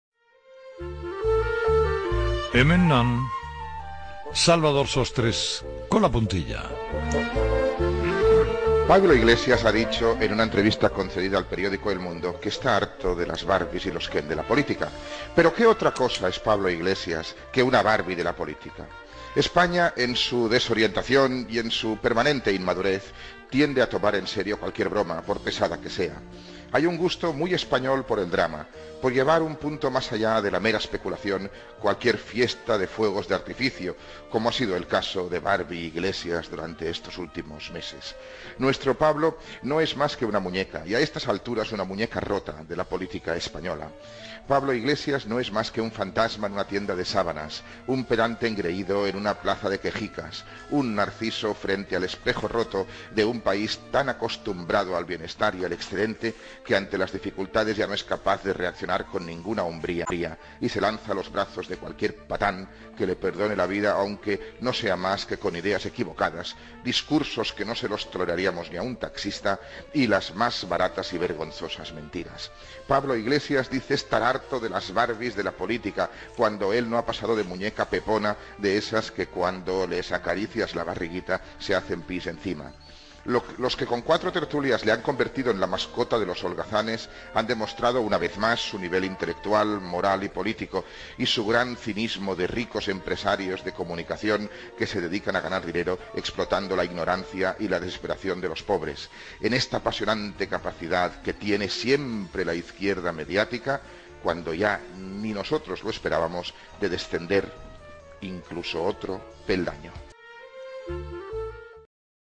Salvador Sostres Dedica "La Puntilla" a Pablo Iglesias" | La Barbie de la política (17/11/2015)
Pablo Iglesias ha dicho en una entrevista concedida al periódico EL MUNDO que está harto de las Barbies y los Ken de la política. ¿Pero qué otra cosa es Pablo que una Barbie de la política?

España en su desorientación, y su permanente inmadurez, tiende a tomar en serio cualquier broma, por pesada que sea. Hay un gusto muy español por el drama, por llevar un punto más allá de la mera especulación cualquier fiesta de fuegos de artificio, como ha sido el caso de Barbie Iglesias durante los últimos meses.

Nuestro Pablo no es más que una muñeca, y a estas alturas una muñeca rota, de la política española. Pablo Iglesias no es más que un fantasma en una tienda de sábanas, un pedante engreído en una plaza de quejicas, un Narciso frente al agrietado espejo de un país tan acostumbrado al bienestar y al excedente, que ya no es capaz de sobreponerse a las dificultades con ninguna hombría, y se lanza a los brazos de cualquier patán que le perdone la vida aunque sea no más que con ideas equivicadas, discursos que no se los toleraríamos ni al taxista, y las más baratas y vergonzosas mentiras.

Pablo Iglesias dice estar harto de las Barbies de la política cuando él no ha pasado de muñeca pepona de esas que cuando les acaricias la barriguita se hacen pis encima. Los que con cuatro tertulias le han convertido en la mascota de los holgazanes, han demostrado una vez más su nivel intelectual, moral y político; y su gran cinismo de ricos empresarios de la comunicación que se dedican a ganar dinero explotando la ignorancia de los pobres, y su desesperación, en esta apasionante capacidad que tiene siempre la izquierda mediática -cuando ya ni nosotros lo esperábamos- de descender un peldaño más.